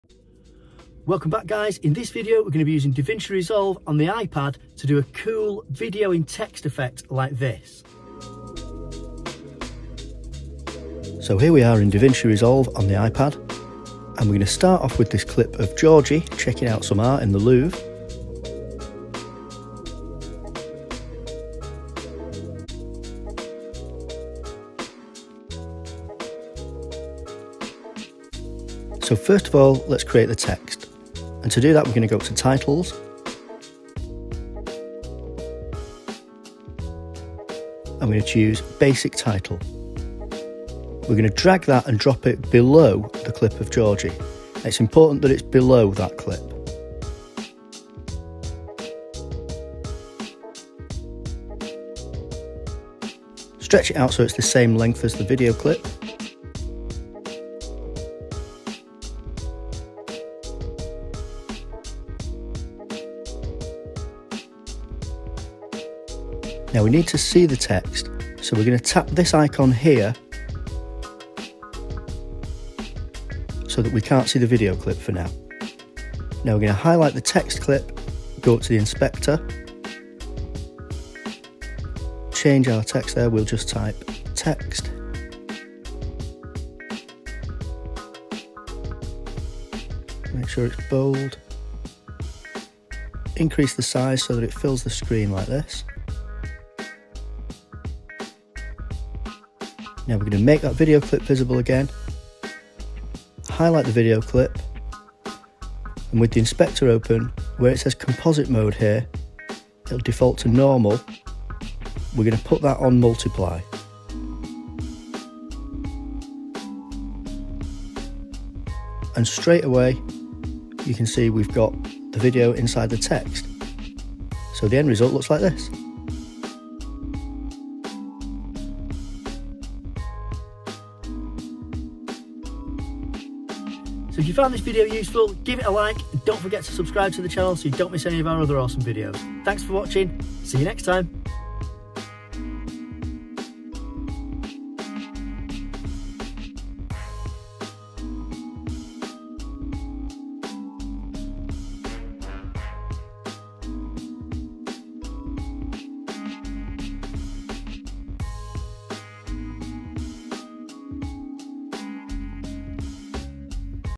How To Place VIDEO IN TEXT // DaVinci Resolve iPad Tutorial
In this video we check out a super simple way of placing video inside text using DaVinci Resolve on the iPad.